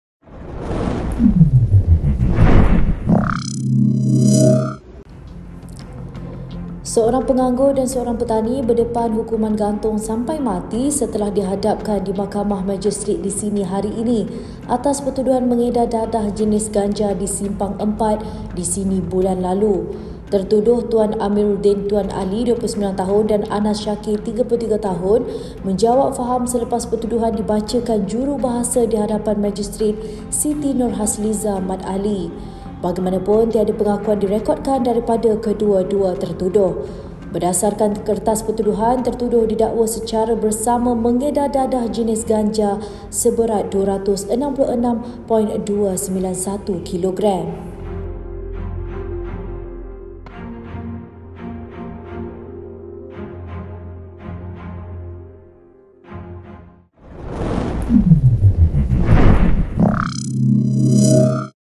Penganggur, petani edar dadah berdepan hukuman gantung
Seorang penganggur dan seorang petani berdepan hukuman gantung sampai mati setelah dihadapkan di Mahkamah Majistret di sini hari ini atas pertuduhan mengedar ganja di Simpang Empat, di sini bulan lalu.